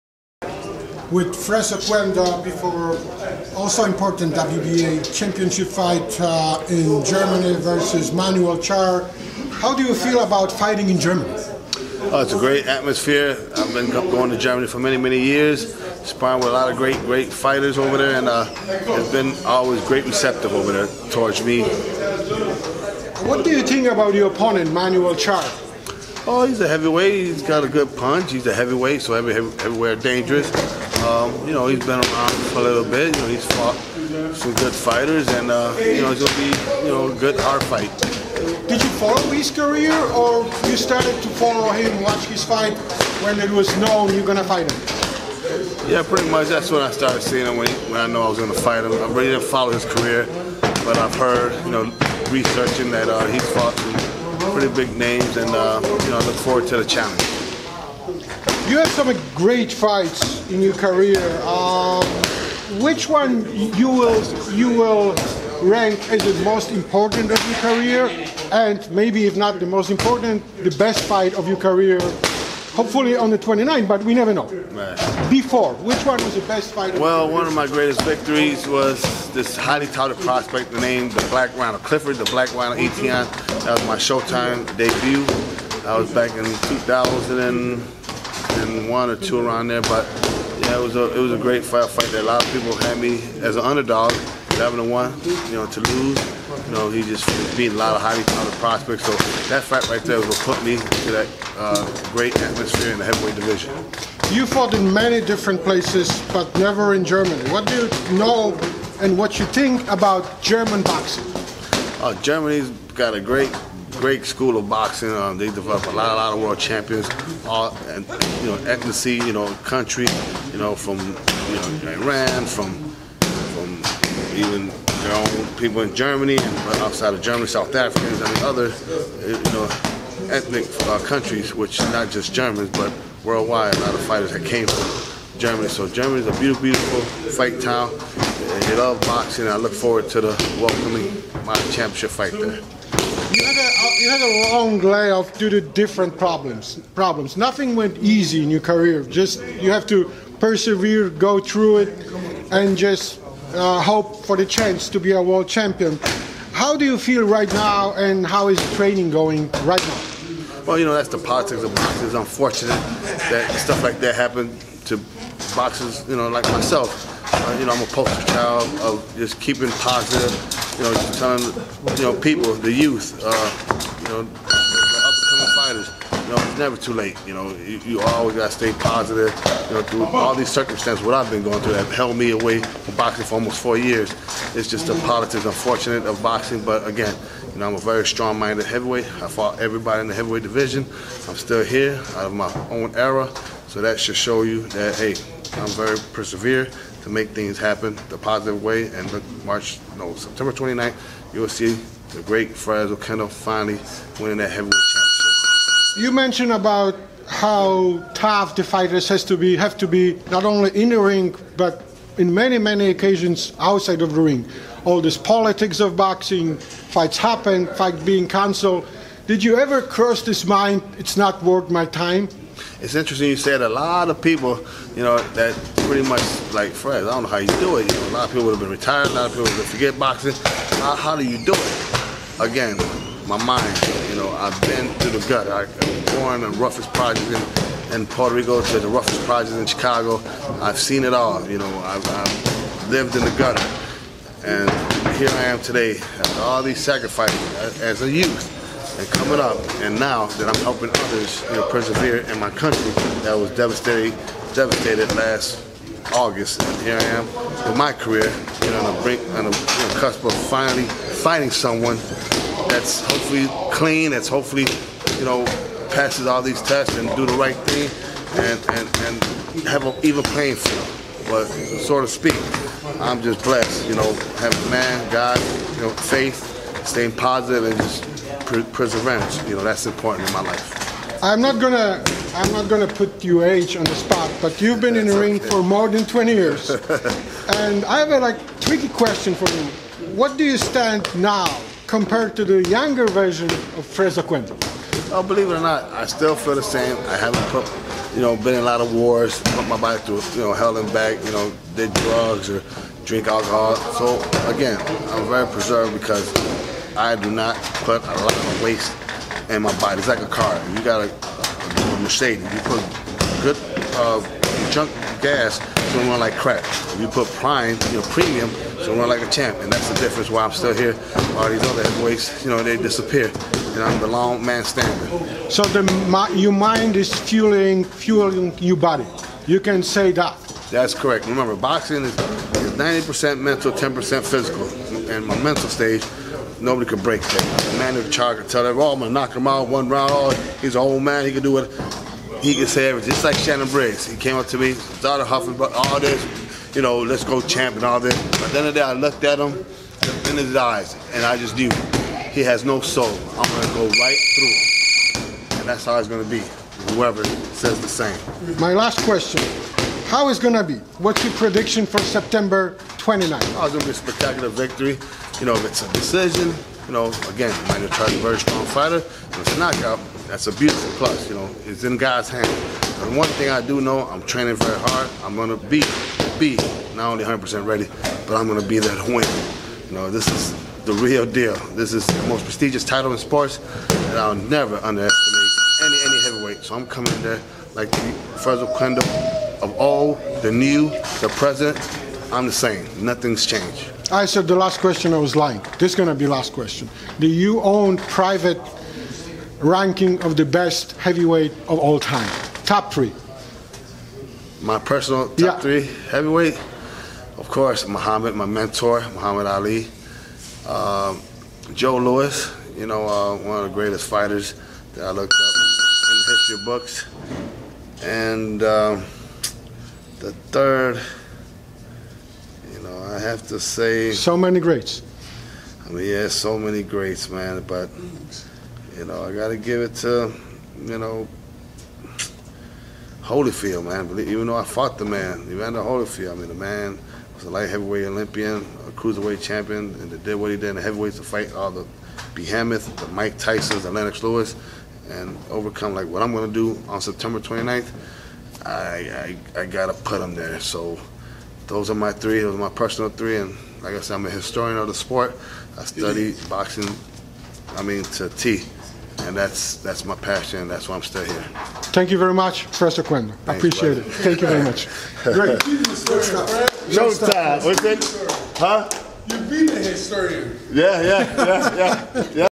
Fres Oquendo: "This is my chance, this is my life"!
Manuel Charr vs. Fres Oquendo, September 29 in Cologne, Germany,